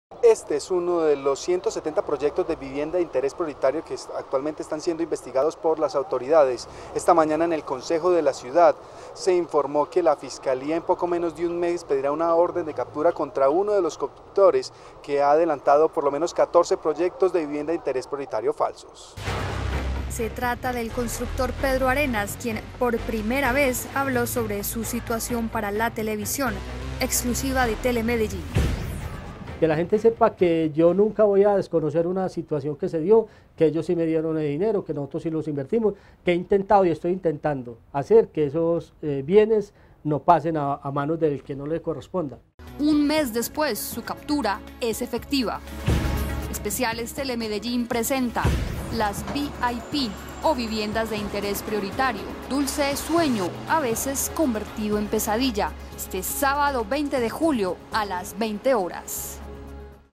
VIP o Viviendas de Interés Prioritario: dulce sueño, a veces convertido en pesadilla
Exclusivo Telemedellín .

Especiales Telemedellín tuvo contacto directo con Pedro Arenas, constructor capturado el jueves 18 de julio en la ciudad de Medellín; persona sobre la cual cursan ante la Fiscalía 143 denuncias por fraude en el tema de las llamadas falsas viviendas de interés prioritario (VIP). Un tema álgido que hoy convoca el interés de la ciudadanía.

Sábado a las 8:00 p.m., con repetición el lunes a las 10:00 p.m.